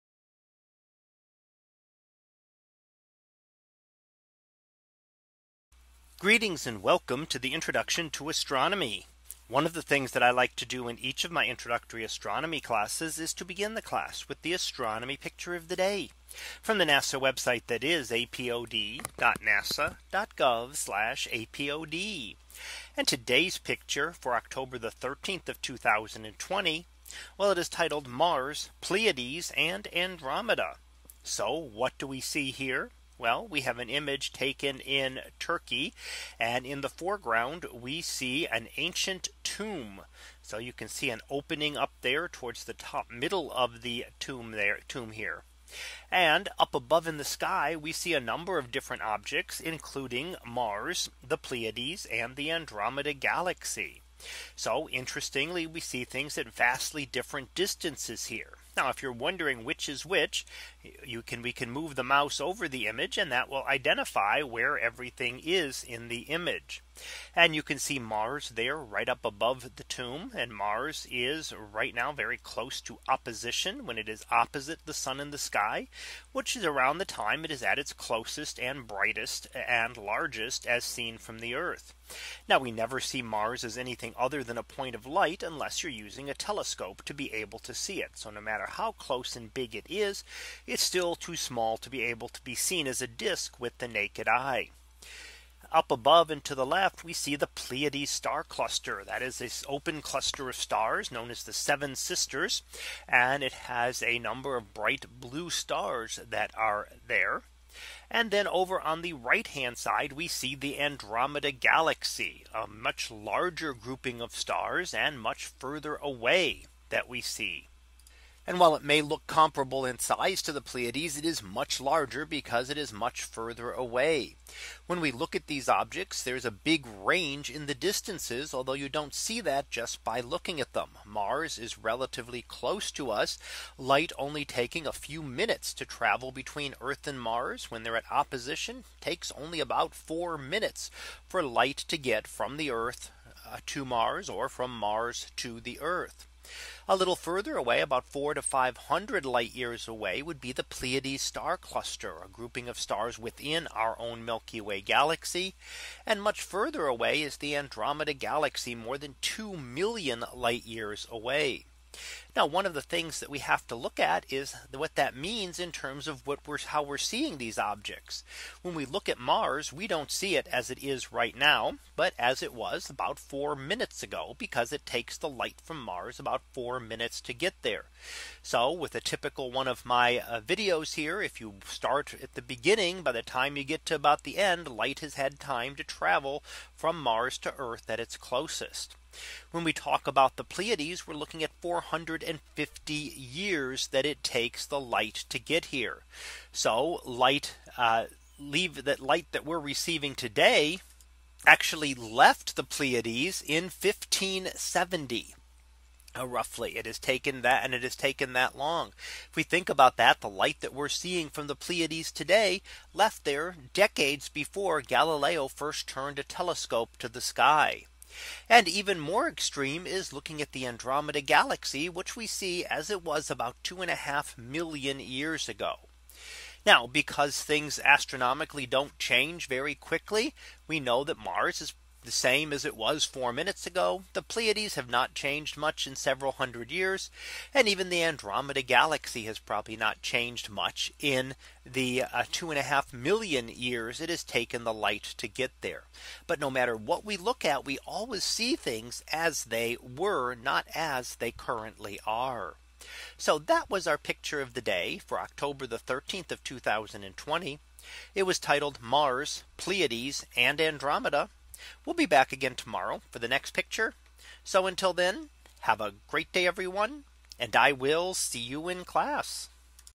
2020 October 13 - Mars, Pleiades, and Andromeda
In today's image, we see an ancient tomb in the foreground and above in the sky we see Mars, the Pleiades star cluster and the Andromeda Galaxy. While they appear together on the sky, they are actually at vastly differing distances from Earth!

I also provide lecture videos to accompany the free OpenStax Astronomy textbook on another channel.